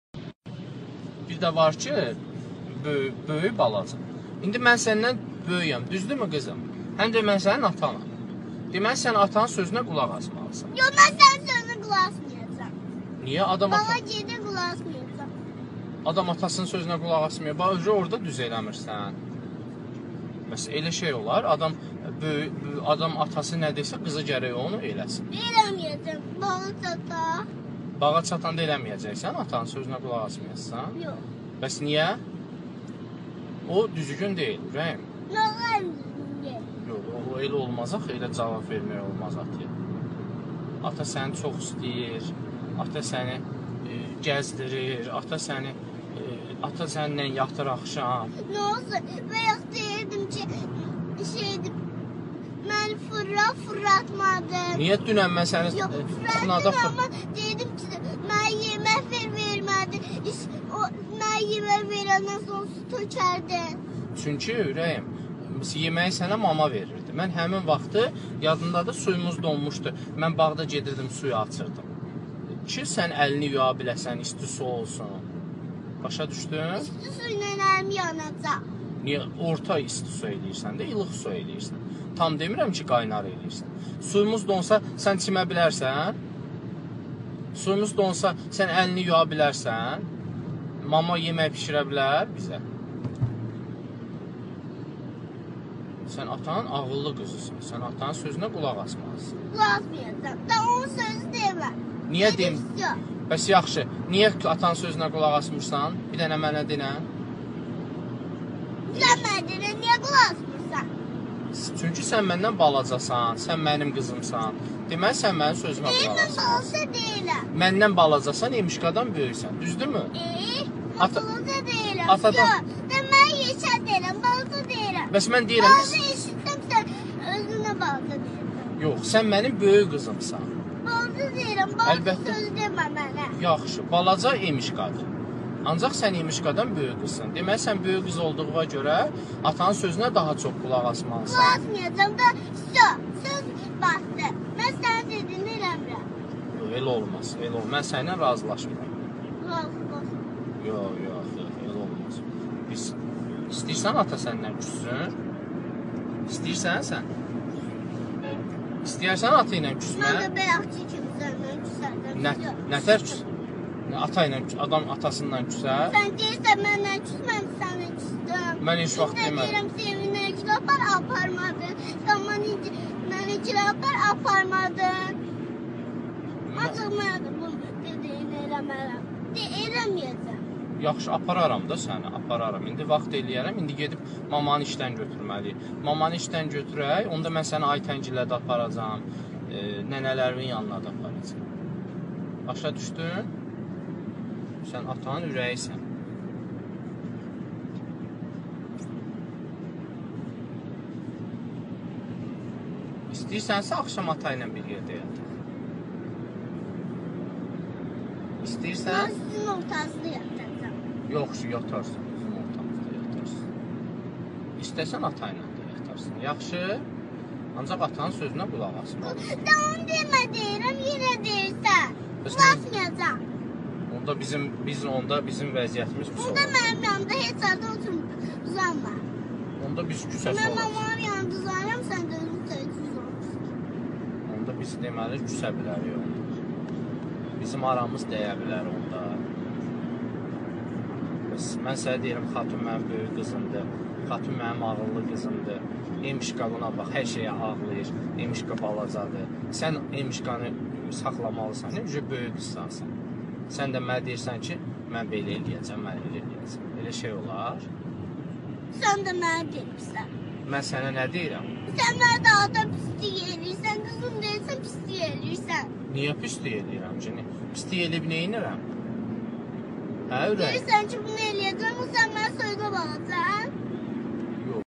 Bakıda Ata və Qızın maşında maraqlı söhbəti 2015
Ata və qızın maşında maraqlı söhbəti